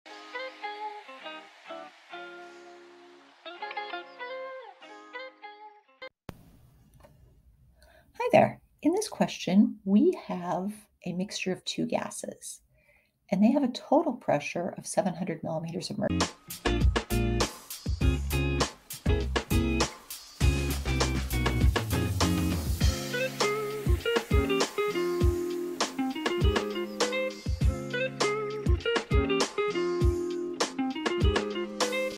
20.0 Liters of a mixture of N2 gas and CH4 gas was made up with a pressure of 700. mm Hg and a temp…
20.0 Liters of a mixture of N2 gas and CH4 gas was made up with a pressure of 700. mm Hg and a temperature of 300. C. If the partial pressure of N2 gas was 100. mm Hg, how many grams of CH4 were added to the mixture?